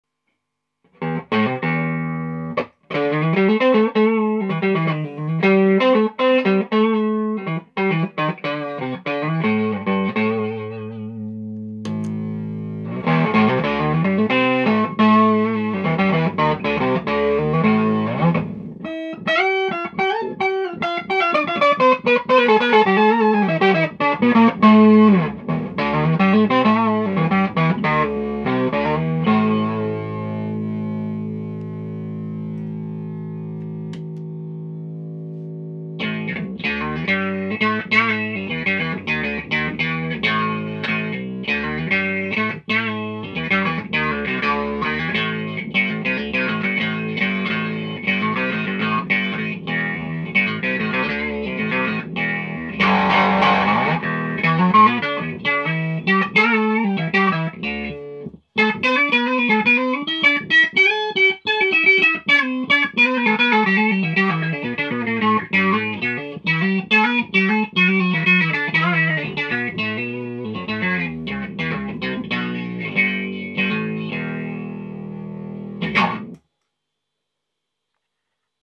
Madbean LowRider + Albondiwah! Envelope Filter On Strat!
Madbean LowRider (DIY Pearl Octave) + Albondiwah! Envelope Filter (DIY Lovetone Meatball).